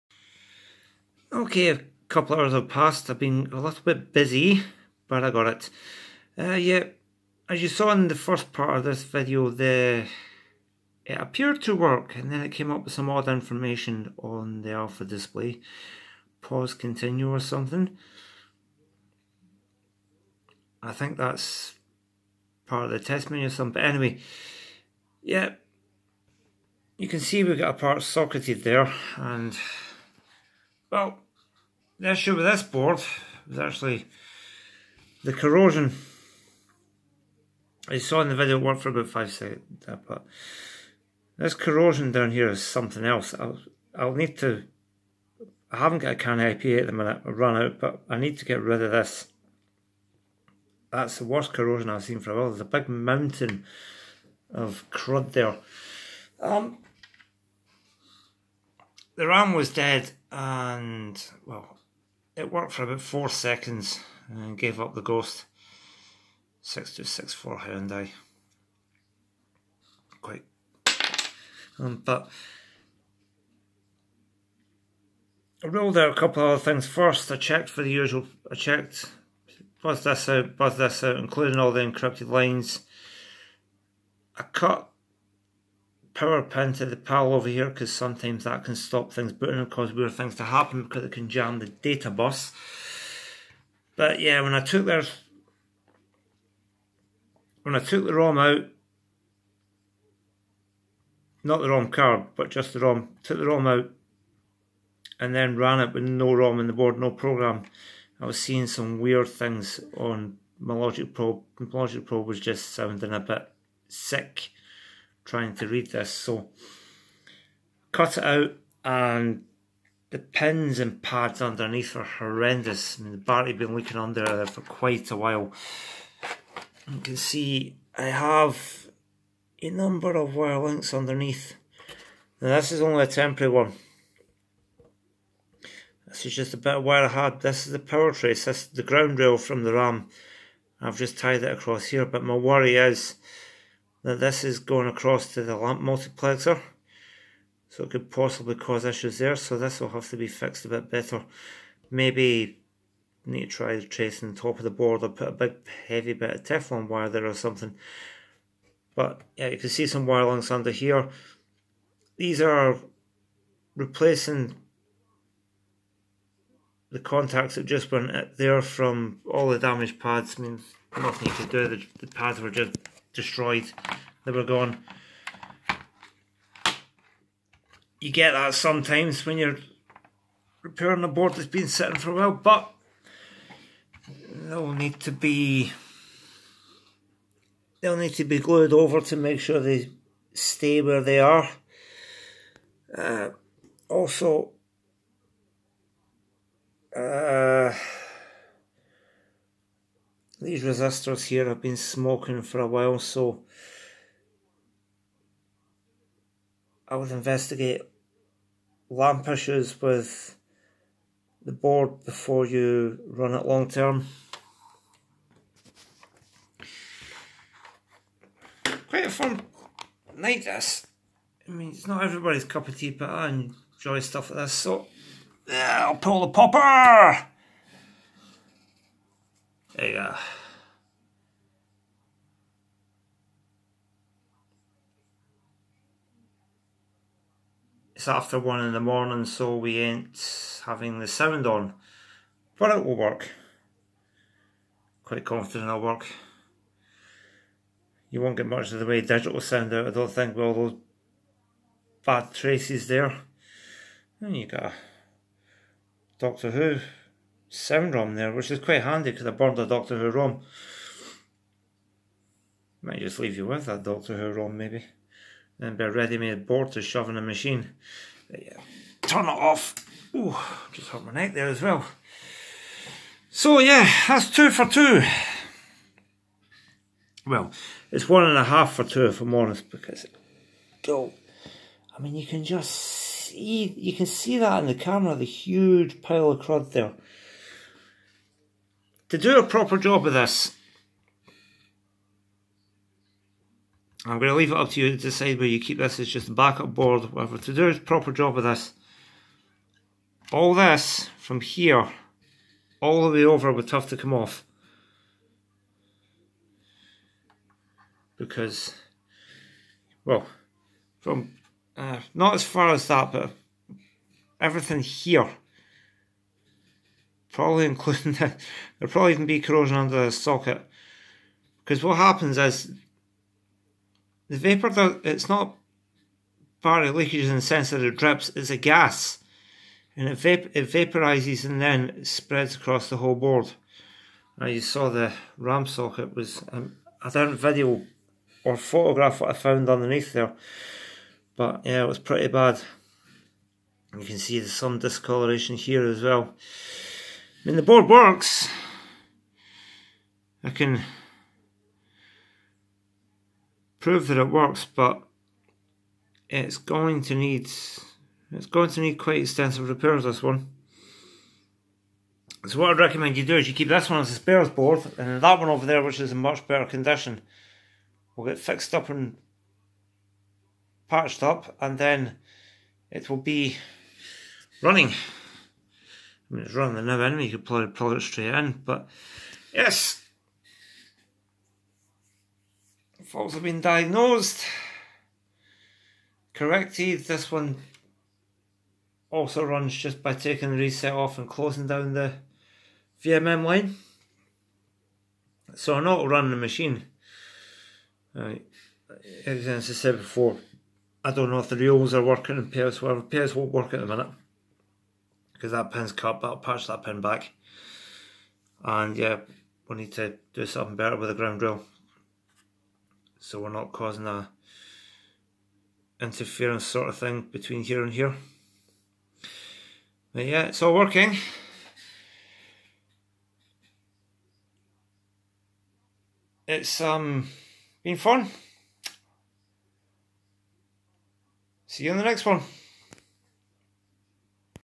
Second Hartley board alive!
Still has a lot of nasty corrosion and heat damage but the second board is ressurected by replacing the HY6264 and patching up some bad traces.. check out the thermal damage to it tho under the lamp section !!